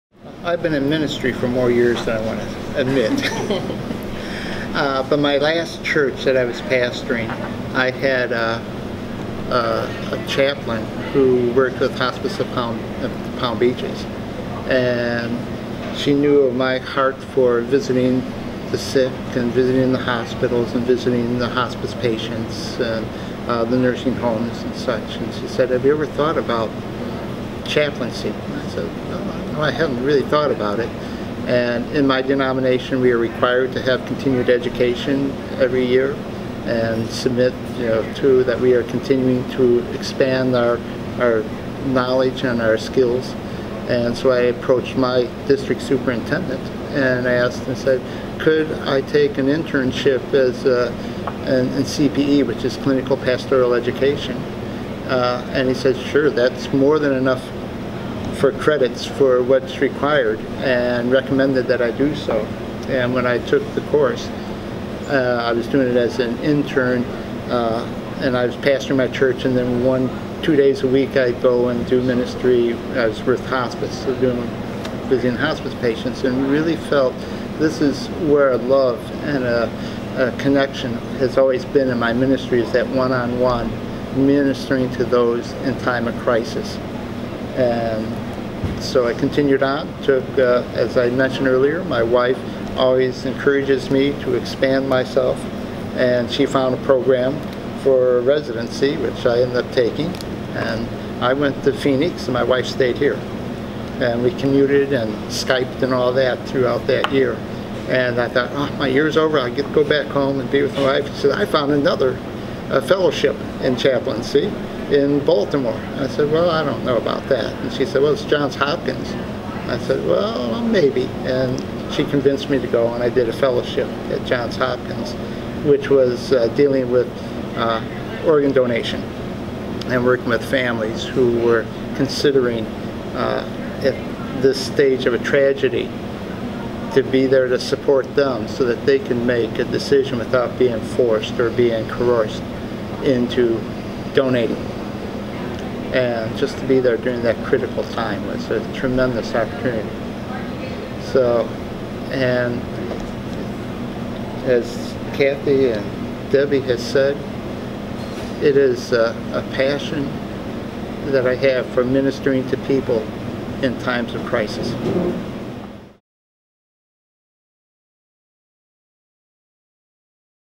What led Rev. David Reeves to the chaplaincy?
Rev. David R. Reeves, Jr. talks about how he became a Chaplain with Martin Health System. Chaplain Reeves is located at Martin Medical Center in Stuart, Florida.